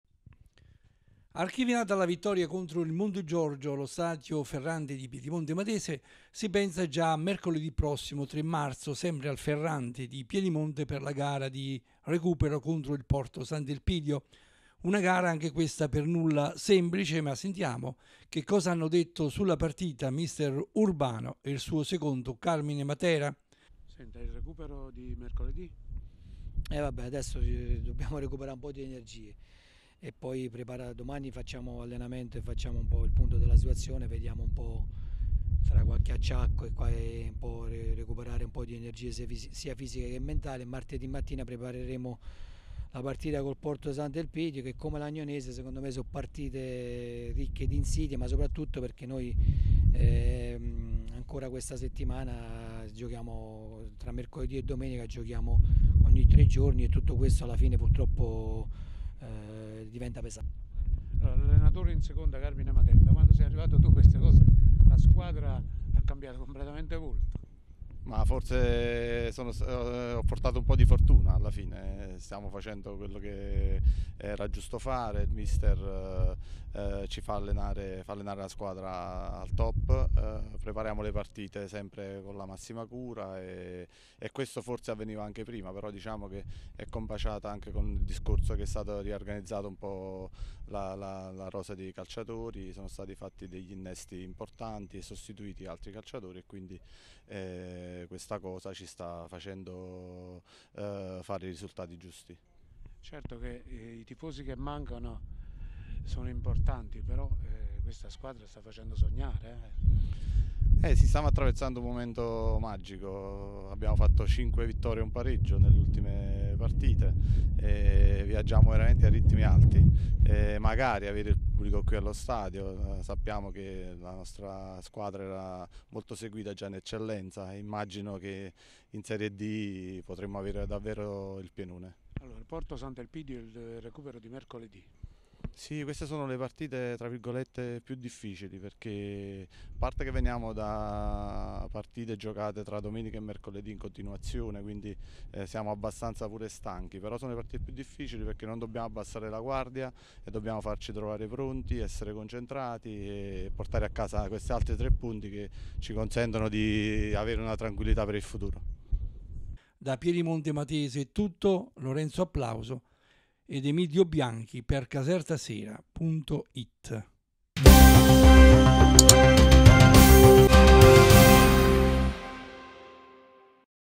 Casertasera.it Urbano e Matera fanno le previsioni in vista del recupero con Porto Sant'Elpidio
L'informazione online della provincia di Caserta.Interviste esclusive a Mr Urbano e Mr.Matera in vista del recupero con il porto sant'Elpidio